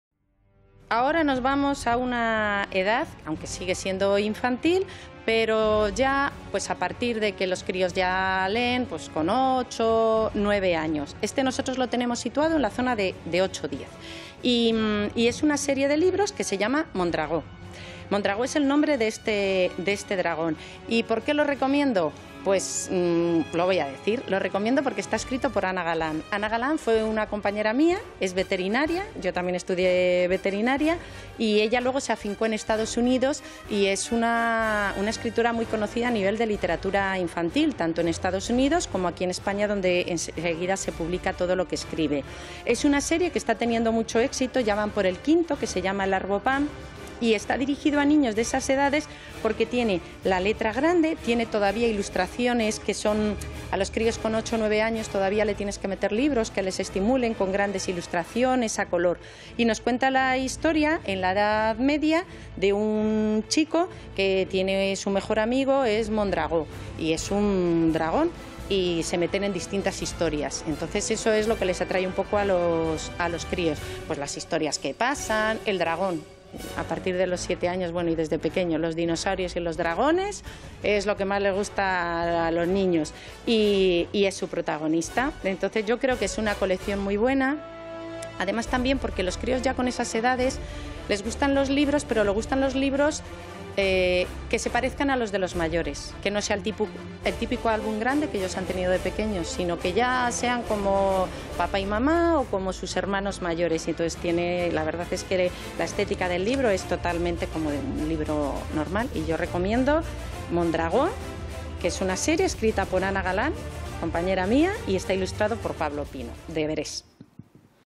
Recomendación Literária
La recomendación literaria de hoy es para los jóvenes; por eso hablaremos de aventuras, nos lo presenta Almudena Pallás de la Librería Taiga.